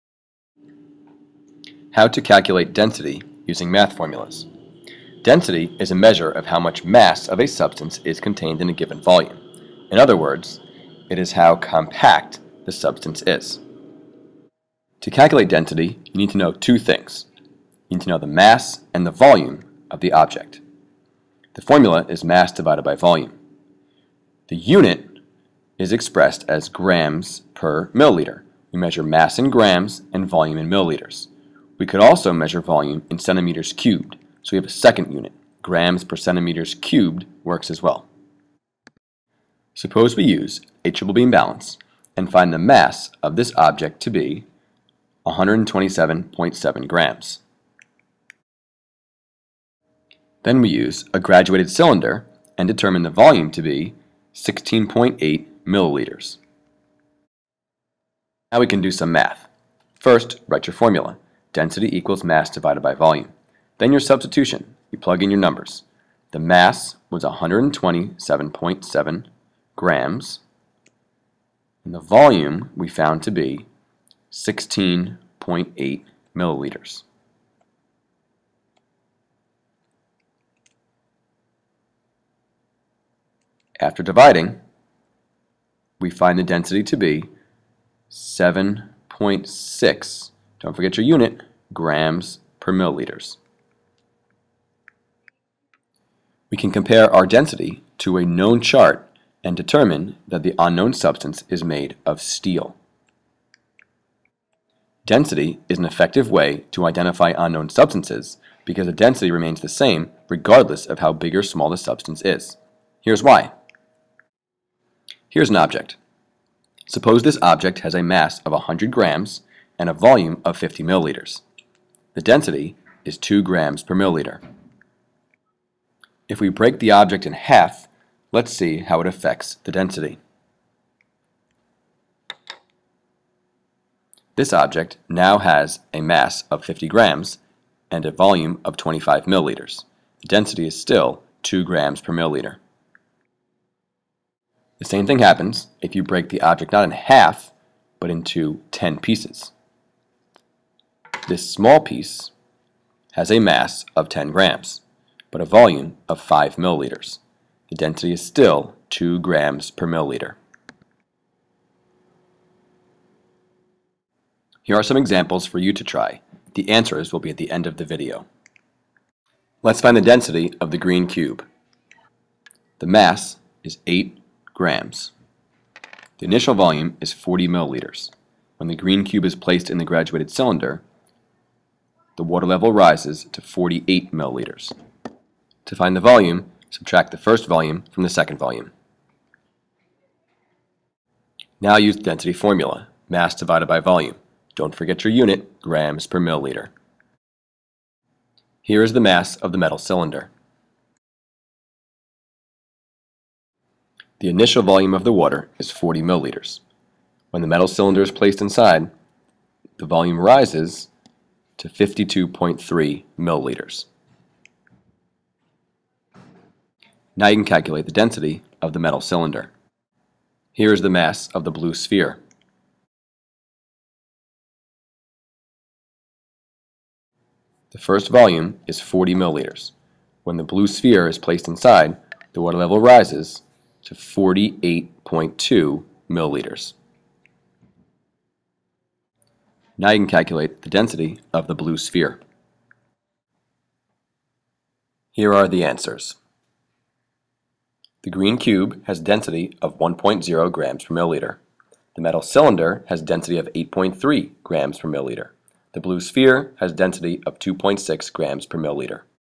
Calculating Density
How to calculate the density of a substance when given the mass and volume.